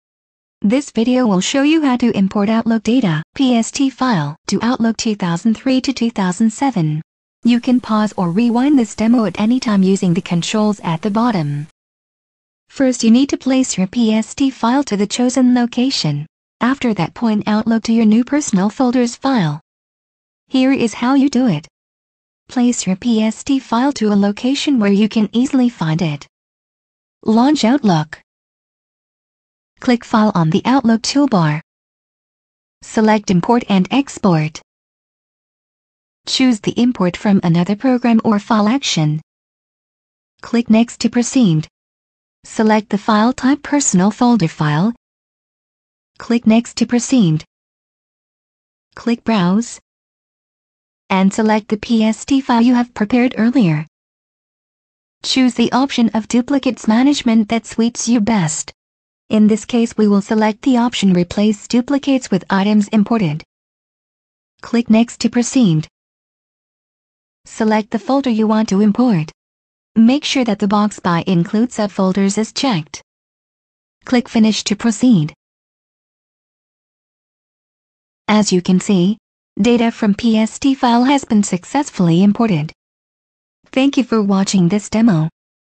How to import Outlook data (PST file) to Outlook 2003 - 2007
- Import Outlook data (PST file). Restore Outlook data in case it was lost or damaged. Outlook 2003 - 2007.